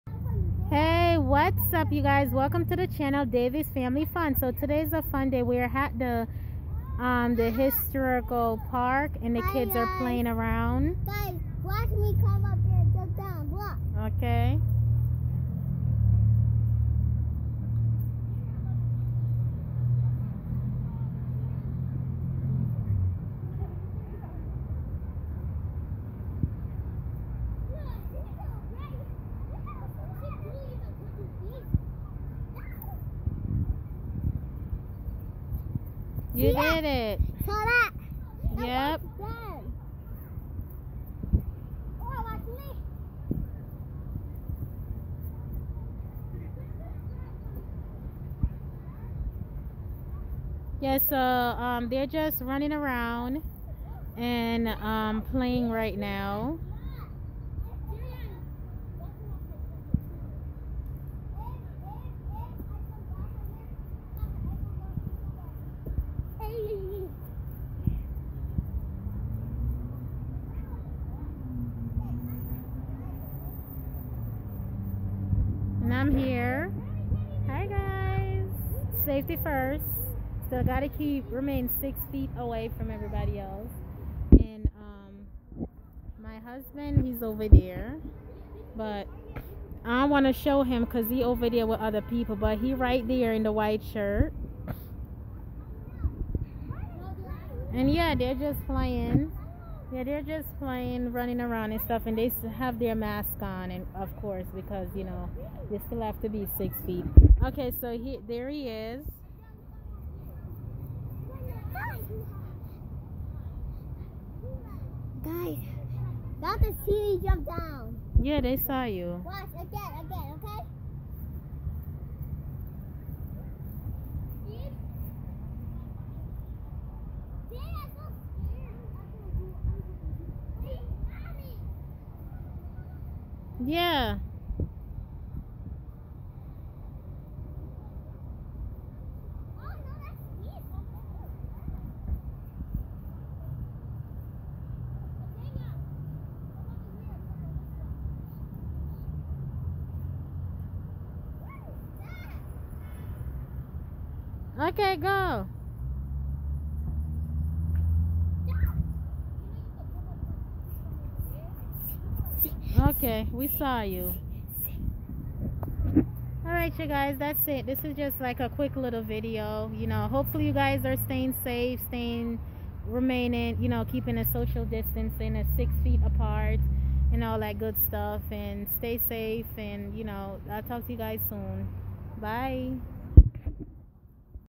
family day at the historic great falls 😊🥰 the kids play and have fun
hey families
I recorded this video 1 months ago but forgot to upload it, the kids having fun at the great falls historical waterfalls.

Davisfamilyfun